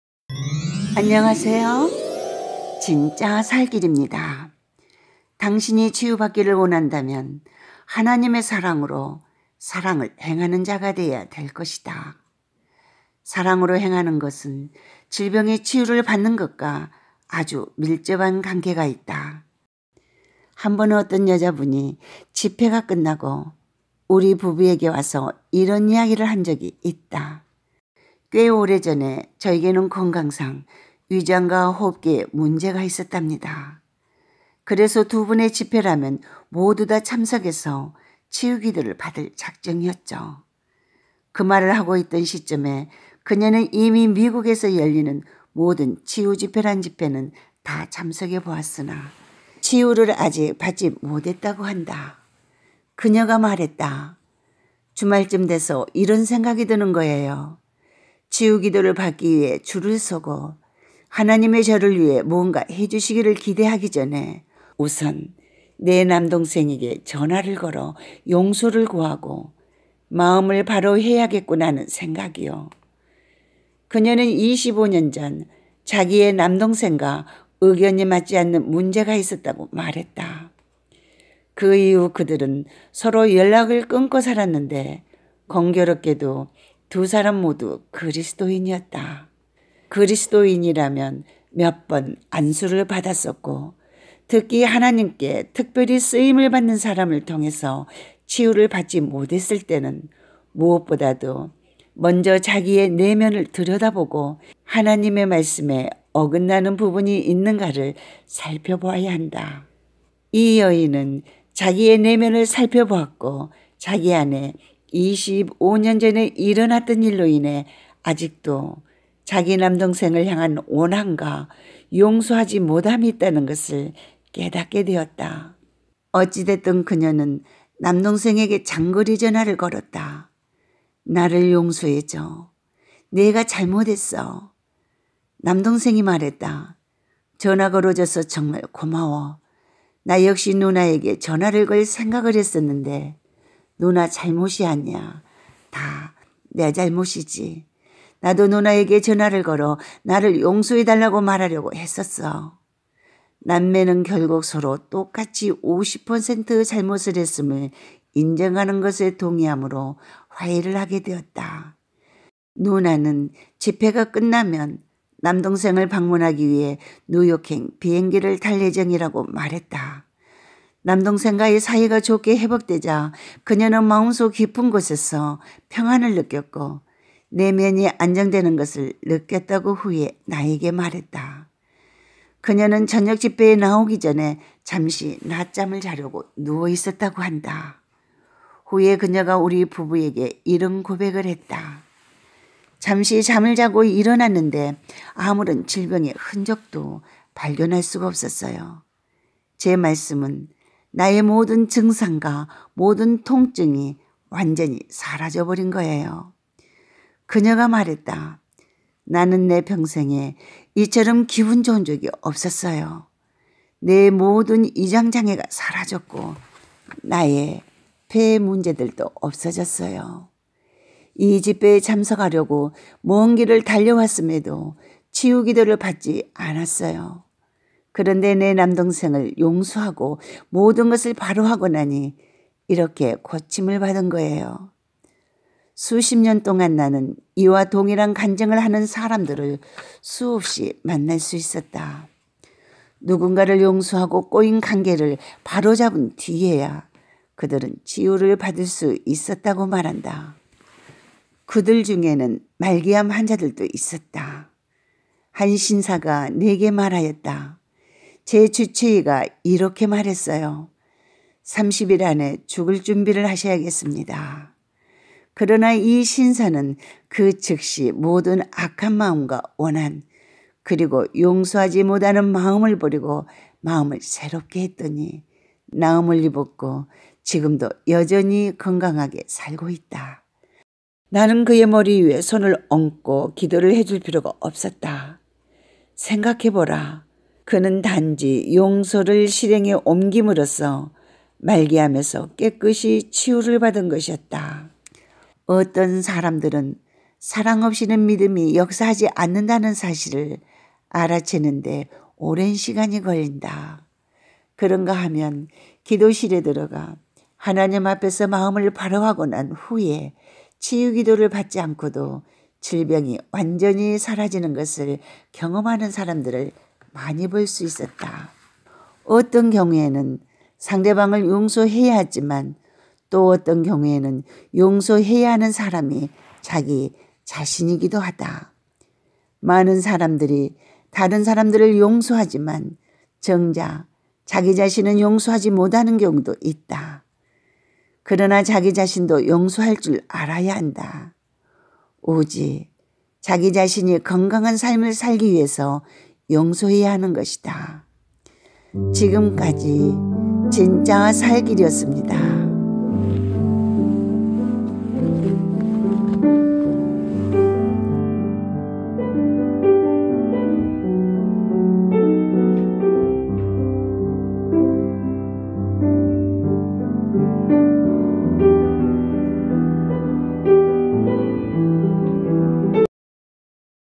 말기암 환자가 수술없이 치유 받다 케네스 해긴 목사님 간증
용서를 통해 말기암 환자가 수술없이 치유 받는  케네스 해긴 목사님 간증.
한 신사에게 주치의가
말기암으로 인해 30일 안에 죽을 준비를 하라는 말을 들었을 때 그는 즉시 모든 악한 마음과 원한을 버리고
남을  용서하고 마음을 새롭게 했더니 수술없이 깨끗하게 치유 받은 놀라운 간증과 그 외의 또 다른 여성은 25년동안 결별하고 있었던 남동생에게 잘못을 구했을 때 오랜 병으로 고통받고 있었던  위 병과 호흡계가  즉각적으로 치유받은 간증입니다.